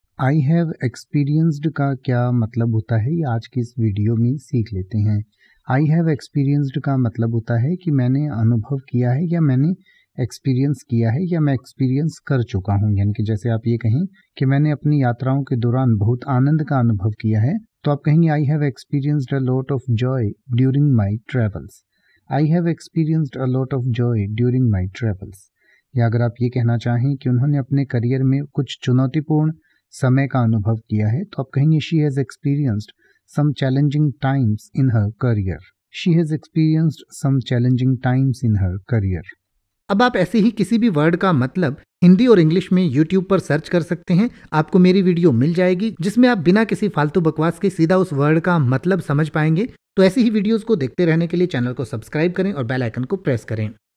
I have experienced meaning in Hindi | I have experienced ka matlab kya hota hai | English to hindi
इस वीडियो में आप I have experienced का हिंदी में मतलब समझेंगे और साथ में I have experienced का pronunciation भी सीखेंगें यानि की आप I have experienced के अर्थ के साथ साथ ये भी सीखेंगें की I have experienced को कैसे बोले या I have experienced को बोलने का सही तरीका क्या है |

तो बस एक मिनट में सीखिए I have experienced को|
Let’s learn Hindi Meaning of I have experienced in detail.

I have experienced का हिंदी में अर्थ होता है
मैंने अनुभव किया है

I have experienced a lot of joy during my travels.
मैंने अपनी यात्राओं के दौरान बहुत आनंद का अनुभव किया है।
She has experienced some challenging times in her career.
उन्होंने अपने करियर में कुछ चुनौतीपूर्ण समय का अनुभव किया है।

I have experienced को इन उदाहरणों से और अच्छे से समझ सकते हैं